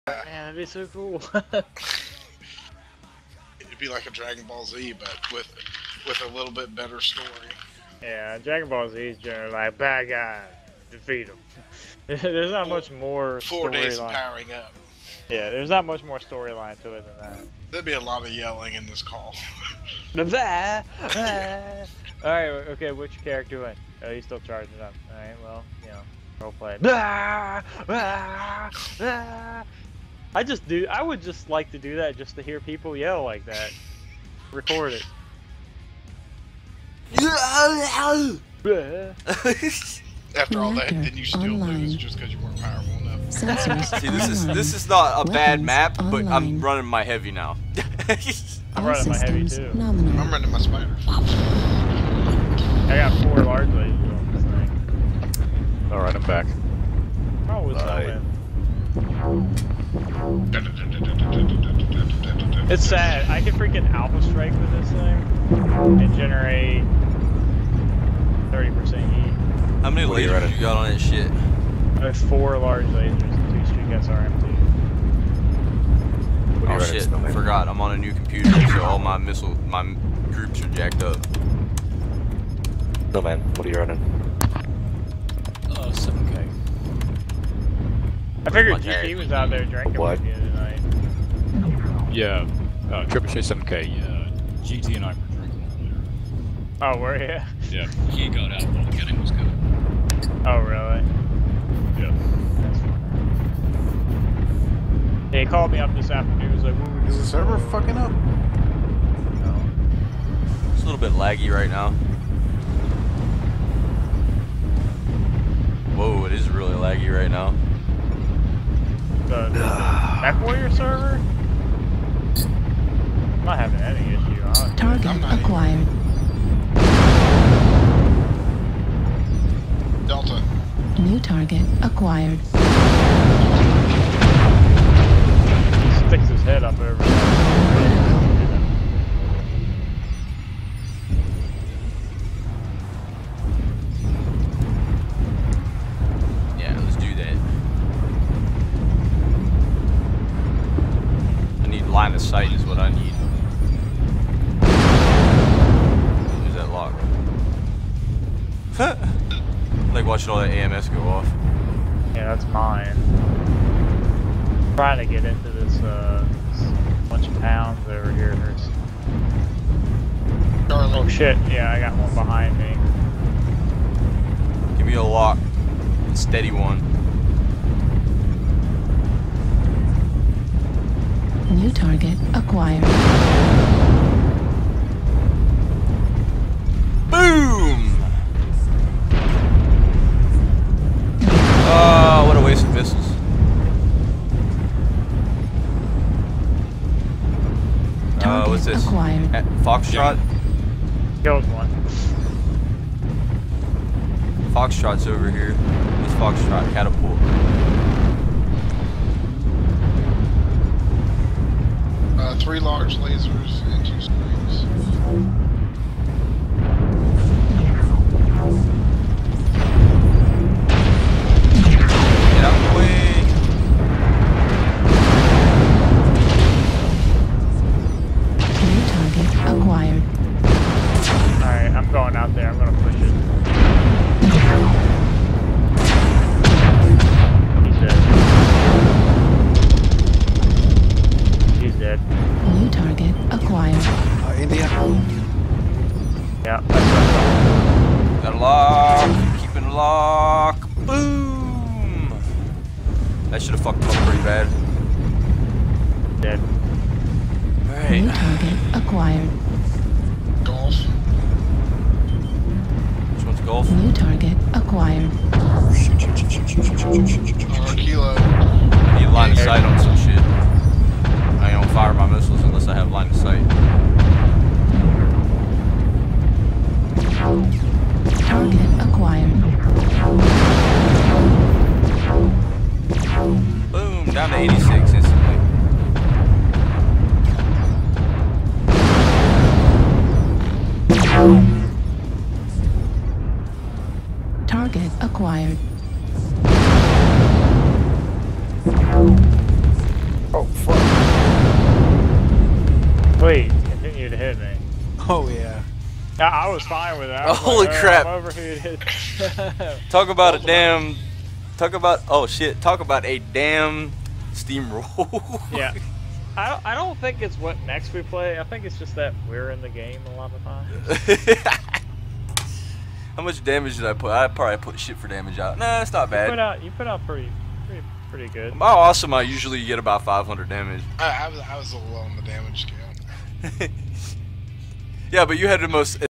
Missile Boating AWS-8R
Running another match with the gents from [SCO]. This time running my missile boat Awesome. This match was a pretty easy steamroll 12-0